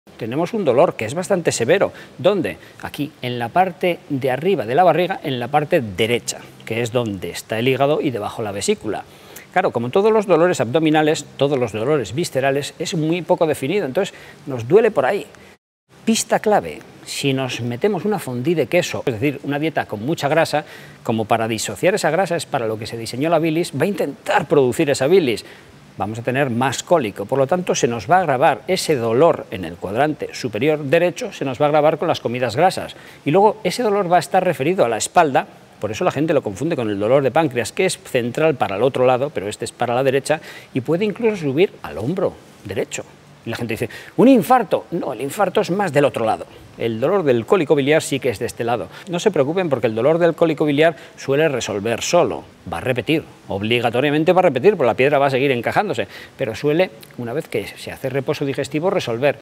¿CÓMO DUELEN las PIEDRAS EN LA VESÍCULA BILIAR?
Las piedras en la vesícula cuando duelen, duelen mucho. Y este dolor aparece y desaparece. Es un dolor que se localiza en unas zonas determinadas del cuerpo que conviene conocer. En menos de 1 minuto descubrirá cómo duelen las piedras en la vesícula biliar.